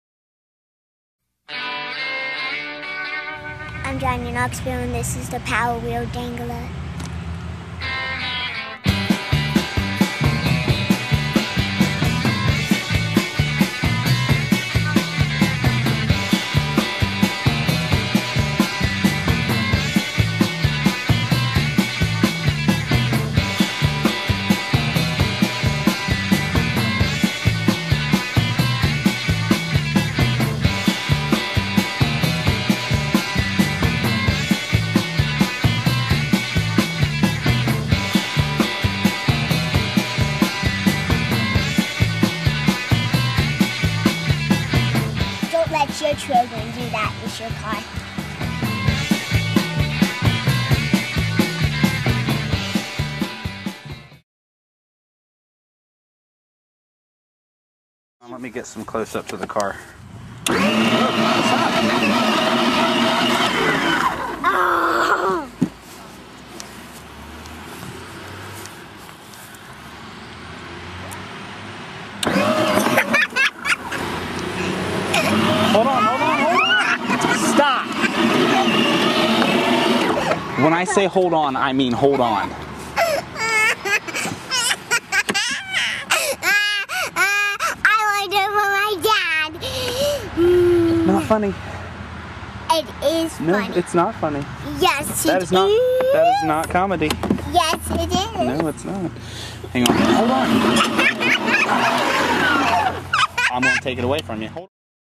Kiddie Jackass (Part 4)
This is one mean PowerWheel, piloted by one mean kid.  STUNTS!